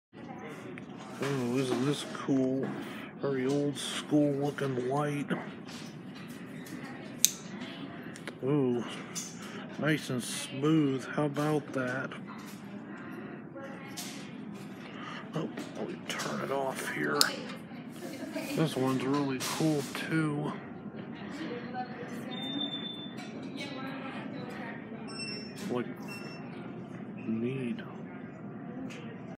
Retro globe lamps at T.J.Maxx
Here are a couple of really neat old school globe lamps at T.J.Maxx. They are really cool looking and a nice touch if you’re into the retro theme.